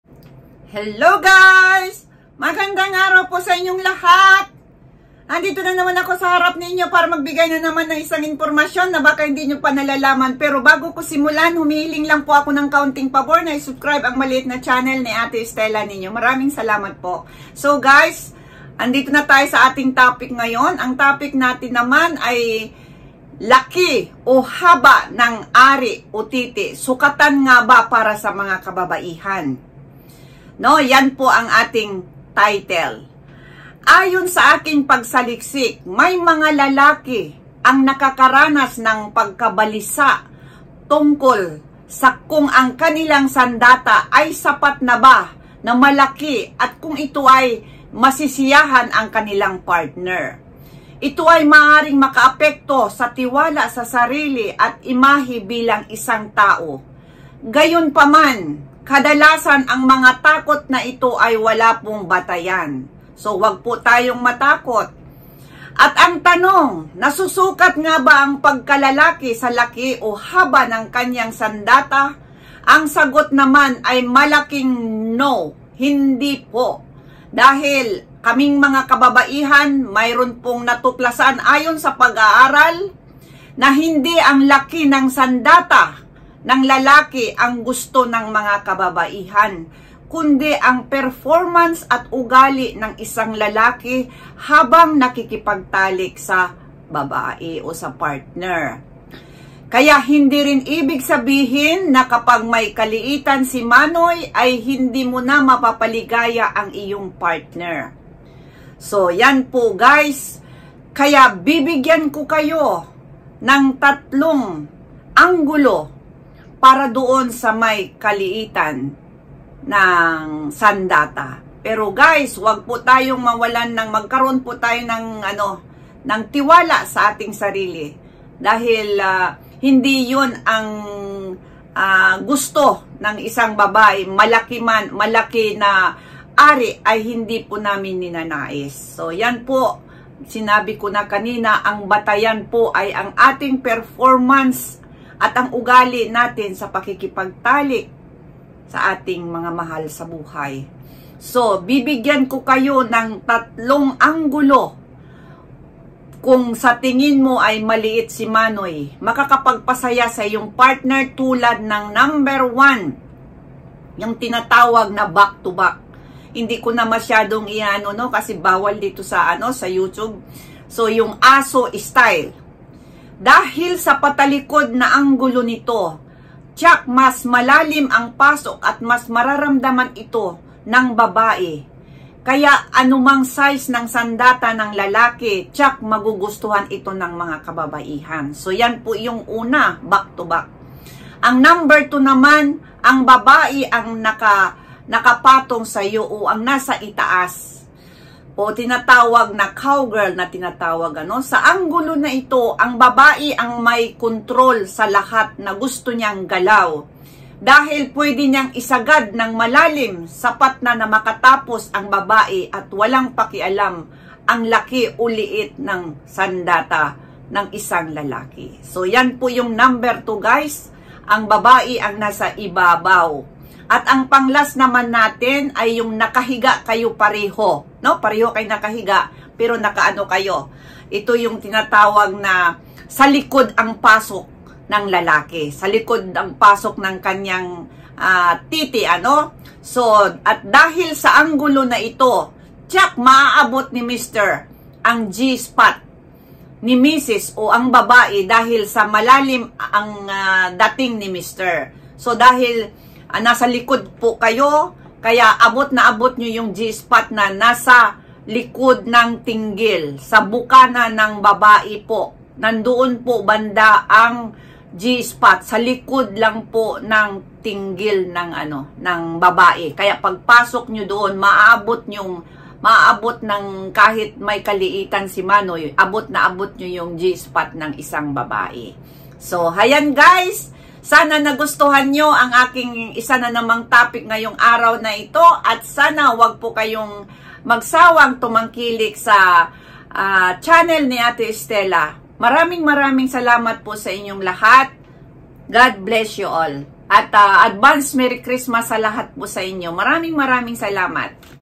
SAAN KA MALA-KI O MATABA
Gusto ba talaga ng isang  babae ang malaking talong? Yan ang tatalakayin natin sa videong ito.